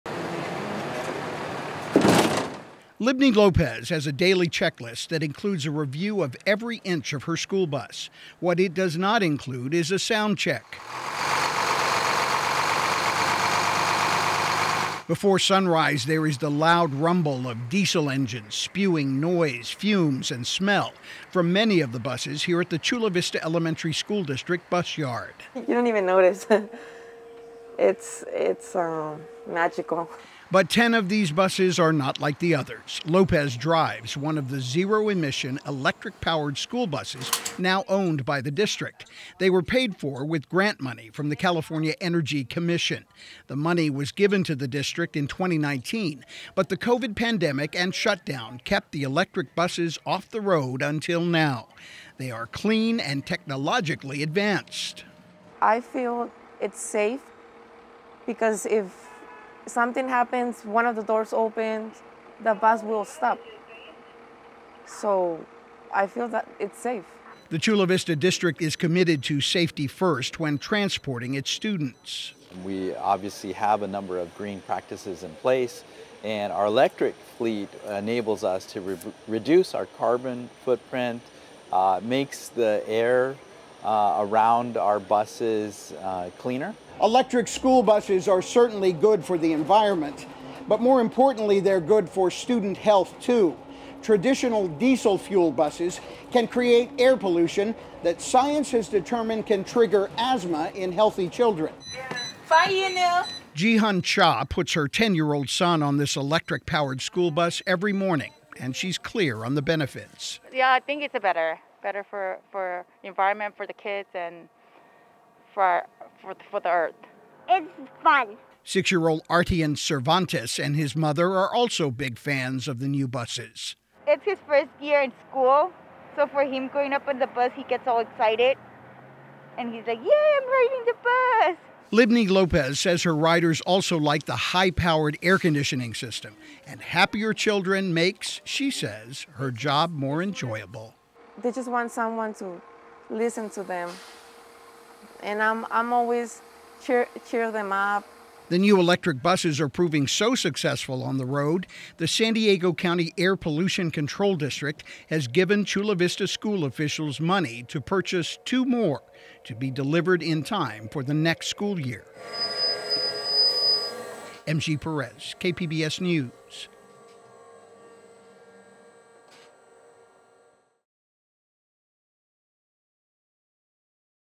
Chula Vista electric school buses charged to help the environment
Some school buses in the Chula Vista Elementary School District are now charged and ready to transport students and save the environment. There are 10-new buses rolling out everyday as the first zero-emission, electric-powered buses in the district’s fleet. KPBS Education Reporter M.G. Perez rode along with one of the bus drivers today.